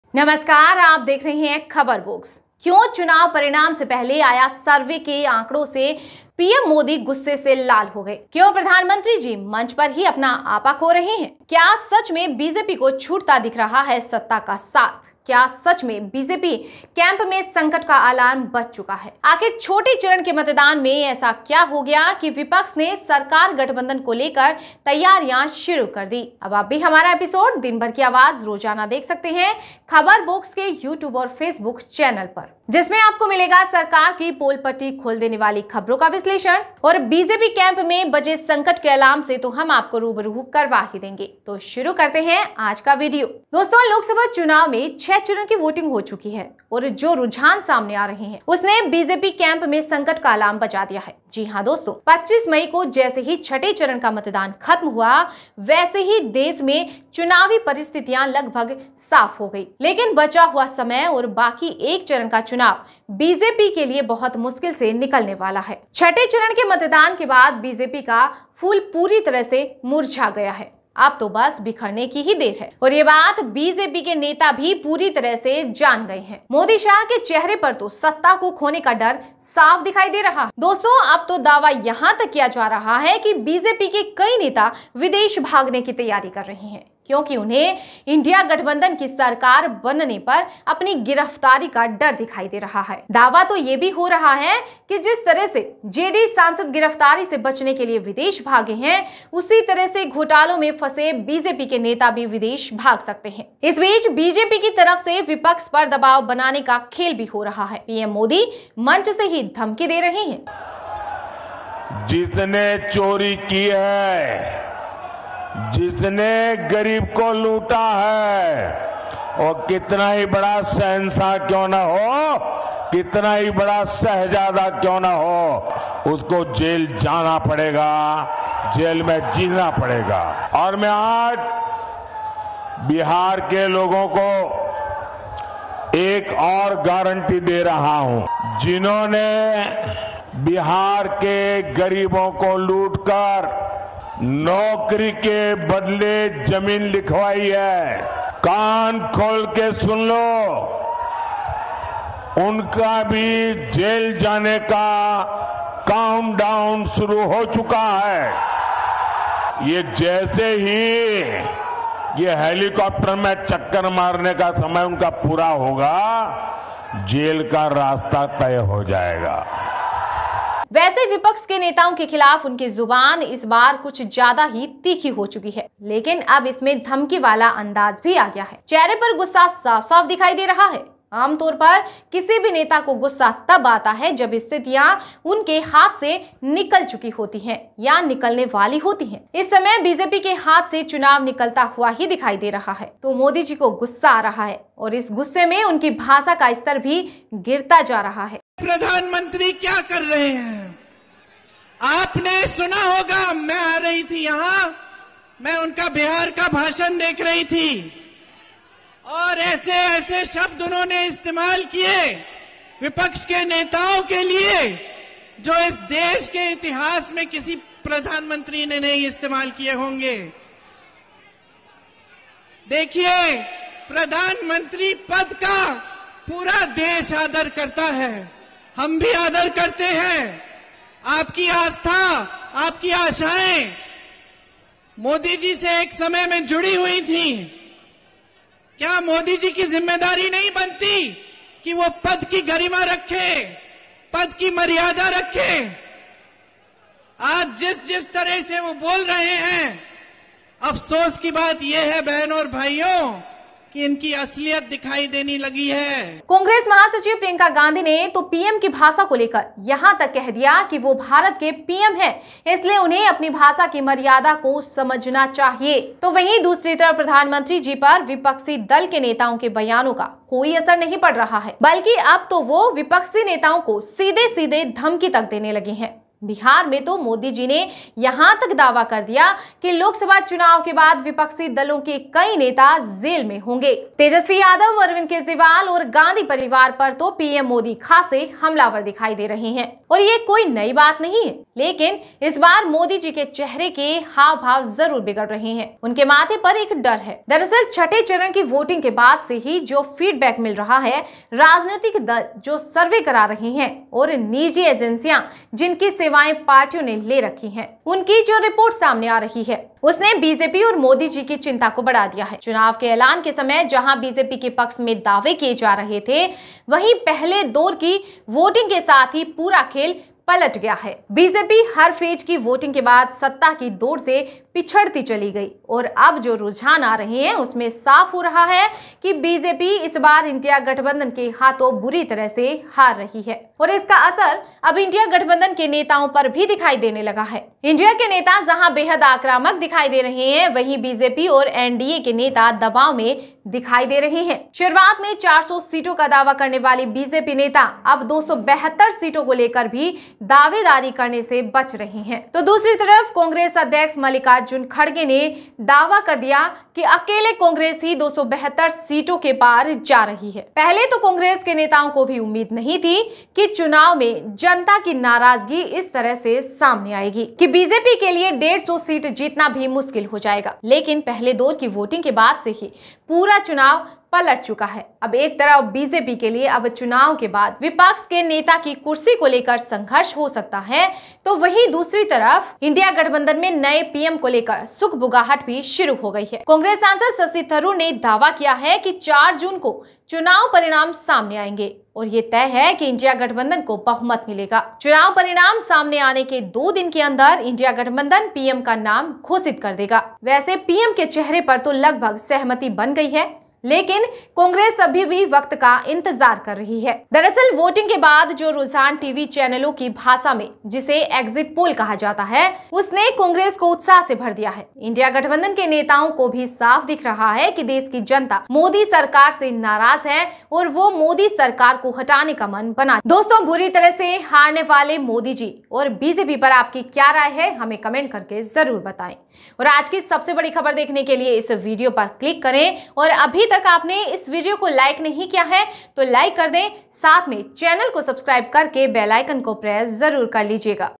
बीजेपी की बुरी हार, छह चरण के बाद गुस्से से लाल हुए मोदी,विपक्ष ने शुरू की सरकार बनानी की तैयारियां
PM MODI,NDA VS INDIA. NDA INDIA BJP INC News, Loksabha Chunaav 2024, Rahul Gandhi Akhilesh Yadav BJP vs Congress Election 2024
क्यों चुनाव परिणाम से पहले आया सर्वे के आकड़ों से ही गुस्से से लाल हो गए मोदी जी। क्यों प्रधानमंत्री जी मंच पर ही अपना आपा खो रहे हैं, क्या सच में बीजेपी को छूटता दिख रहा है सत्ता का साथ, क्या सच में बीजेपी कैंप में संकट का अलार्म बजा चूका है, आखिर छटें चरण के मतदान में ऐसा क्या हो गया कि, विपक्ष ने सरकार गठबंधन को लेकर तैयारियां शुरू कर दी
Topice Cover:-
1.लोकसभा चुनाव 2024 :- लोकसभा चुनाव में छह चरण की वोटिंग हो चुकी है ,अन्तिम चरण की वोटिंग 1 जून को होगी और 4 जून को रिजल्ट जारी किया जाएगा |
2. पार्टी नेताओ का आरोप -प्रत्यारोप :-कांग्रेस महासचिव प्रियंका गांधी ने कहा मोदी भारत के पीएम हैं इसलिए उन्हें अपनी भाषा की मर्यादा को समझना चाहिए,बिहार मोदी जी ने यहाँ तक दावा कर दिया कि लोकसभा चुनावों के बाद विपक्षी दलों के कई नेता जेल में होंगे,  तेजस्वी यादव, अरविंद केजरीवाल और गांधी परिवार पर तो पीएम मोदी खासे हमलावर दिखाई दे रहे हैं |

3..कांग्रेस सांसद का किया दावा  ;-कांग्रेस सांसद शशि थरूर ने दावा किया है कि 4 जून को चुनाव परिणाम सामने आएंगे और इंडिया गठबंधन को बहुमत मिलेगा। चुनाव परिणाम सामने आने के दो दिन के अंदर इंडिया गठबंधन पीएम का नाम घोषित कर दिया जाएगा |